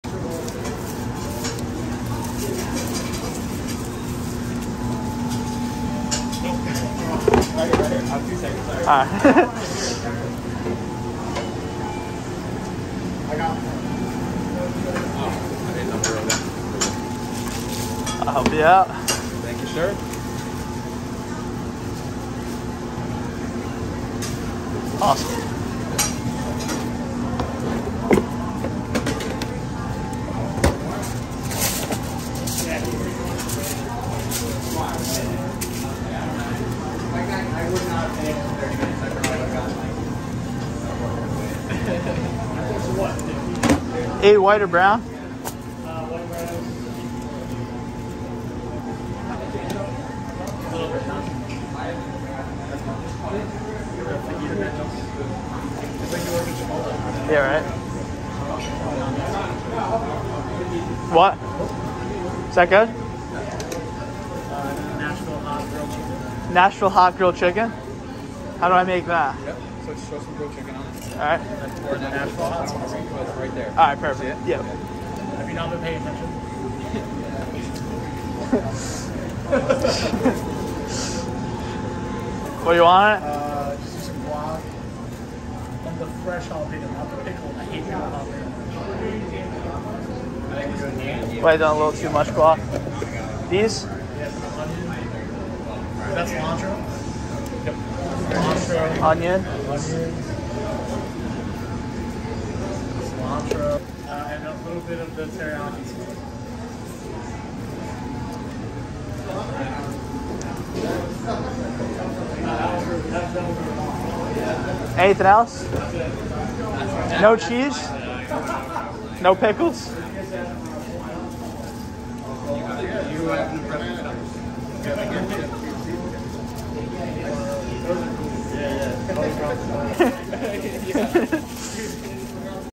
Working at a Burrito Shop
Picked up a shift at a taco shop for the day!